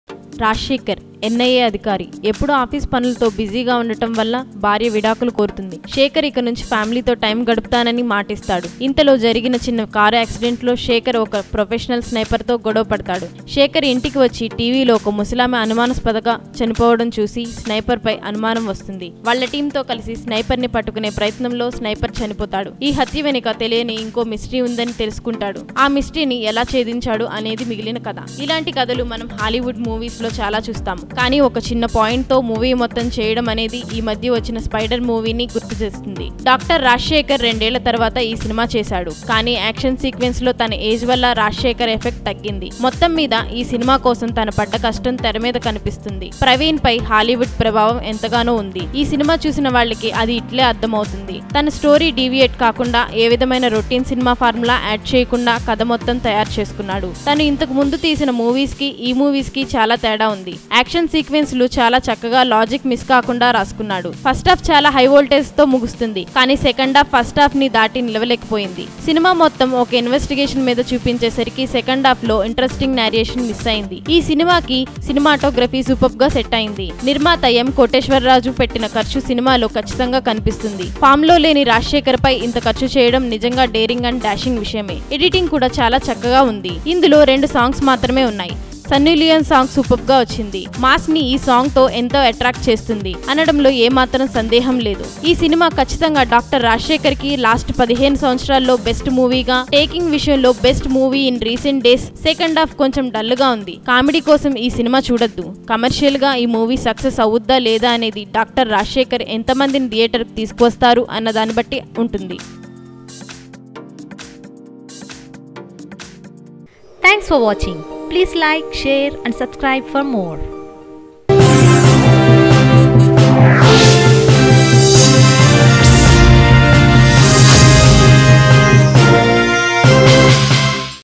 Rajshekhar PSV Garuda Vega Telugu Movie Review | Telugu Mirchi | Suryaa News
Rajshekhar Telugu Movie Review PSV Garuda Vega Superb Response By Public 3 Star rating Please  Subscribes Our Channel  Follow Me and Touch With Latest Telugu Movie News, Movie Trailers and Updates